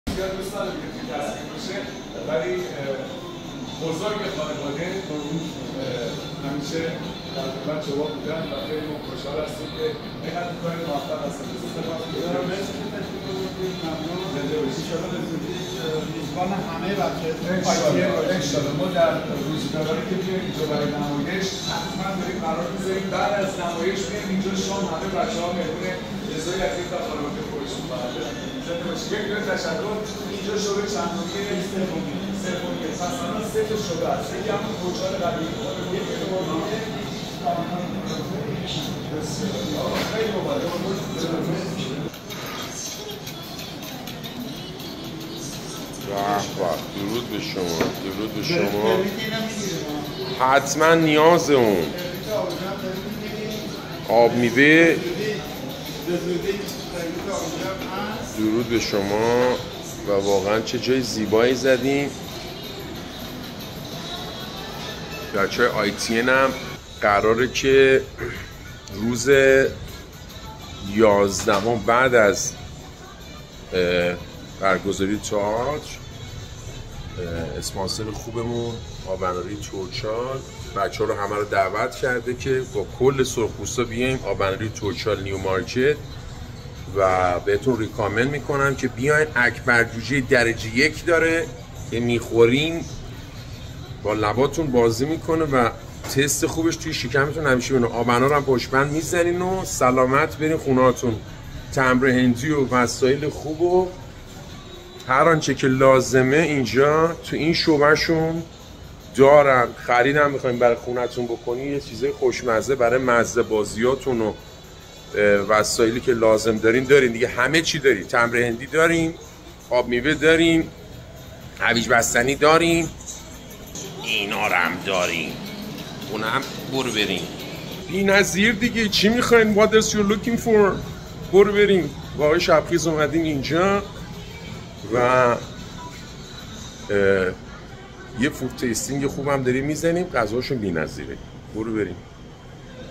افتتاح بزرگ آب اناری توچال با حضور حمید شبخیز - طعمی که هرگز فراموش نخواهید کرد!"
در این ویدیو، ما به همراه حمید شبخیز، مالک و هم‌بنیان‌گذار شبکه تلویزیونی ITN (ایران تی وی نتورک) در لس‌آنجلس، به تولید تبلیغات و برندینگ معروفه توچال پرداختیم. آب اناری توچال به خاطر آب انارهای خوشمزه و تازه‌اش معروف است. در این مکان جدید، می‌توانید از غذاهای لذیذ ایرانی همچون اکبر جوجه با سس‌های خوشمزه انار و هویج بستنی لذت ببرید. همچنین، آب اناری توچال جوزبار متفاوت و خاصی را ارائه می‌دهد که تجربه‌ای متفاوت برای دوستداران طعم‌های ایرانی خواهد بود.در خیابان مین استریت نیومارکت، می‌توانید طعم خوش ایران و توچال تهران را تجربه کنید. این ویدیو شامل نگاهی به فرآیند تولید تبلیغات و معرفی این برند محبوب است.از تماشای این ویدیو سپاسگزاریم." Omid ProductionZ